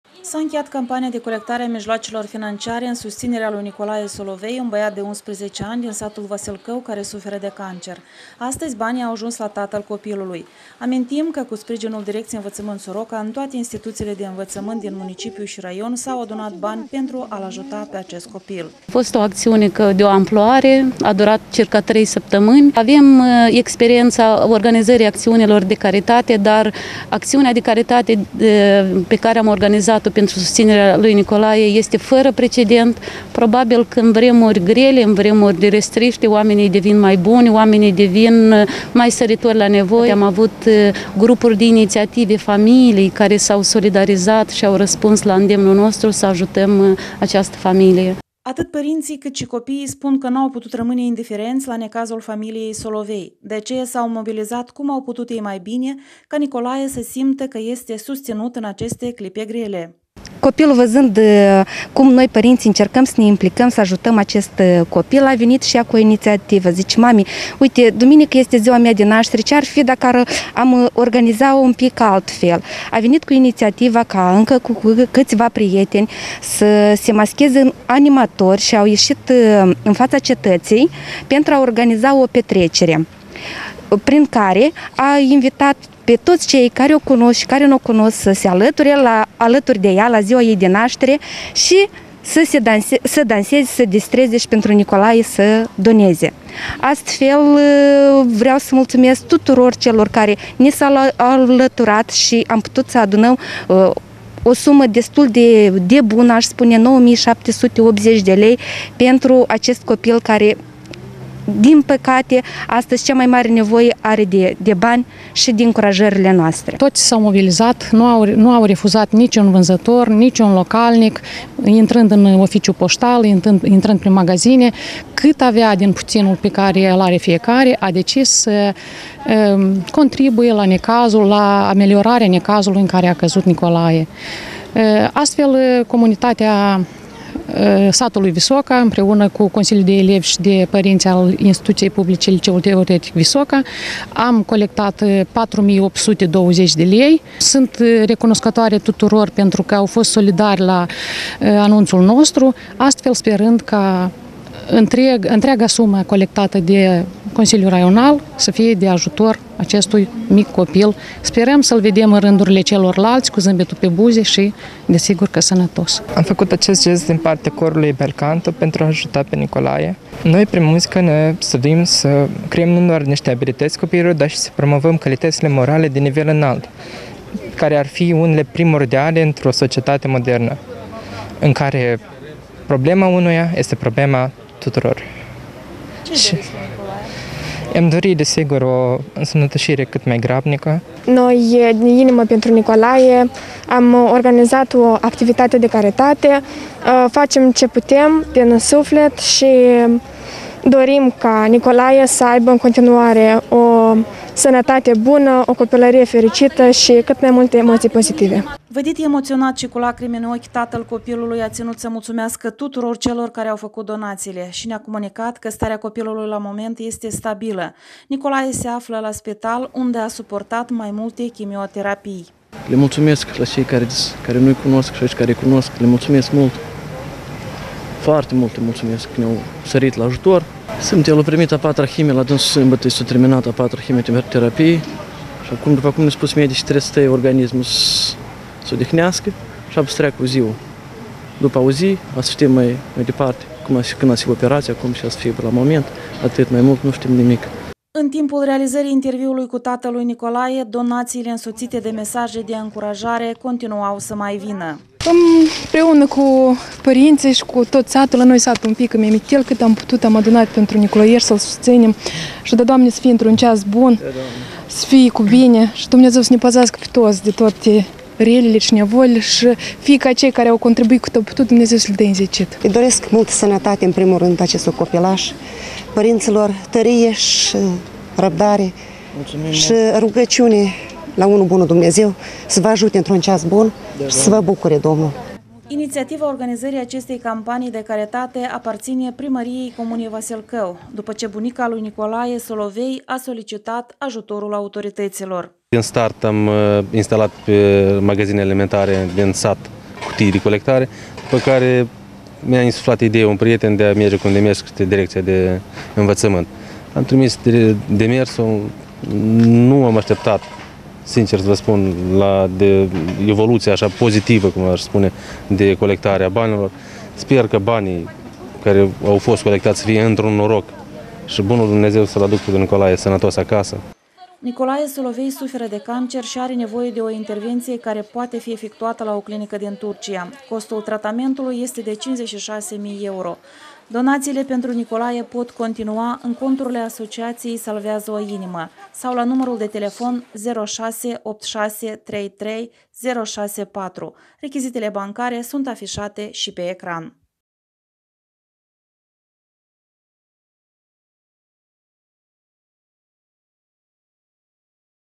S A INCHEIAT CAMPANIA „DIN INIMA PENTRU NICOLAE”
: S-a încheiat campania de colectare a mijloacelor financiare în susținerea lui Nicolae Solovei, un băiat de 11 ani din s. Vasilcău, care suferă de cancer. Astăzi banii au ajuns la tatăl copilului. Amintim că cu sprijinul Direcției Învățământ Soroca în toate instituțiile de învățământ din municipiu și raion s-au adunat bani pentru a-l ajuta pe acest copil.